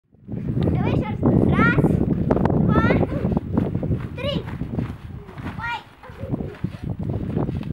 Батут своими руками
Счастливые дети скачут на собранном батуте